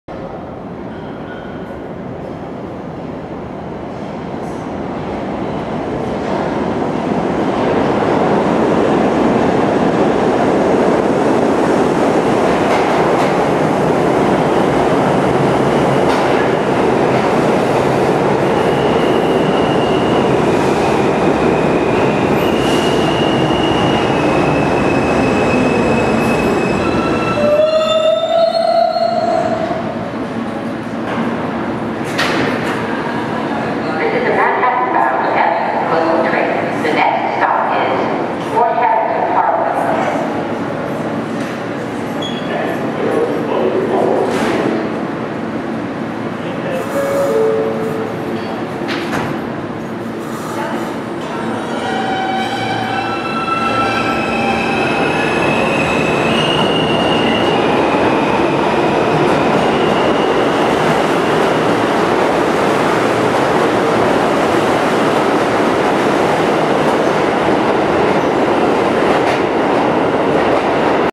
R68 G Train and R160A F Train at Church Avenue (2011)
Lots of action at the G train's (Now Permanent) terminal.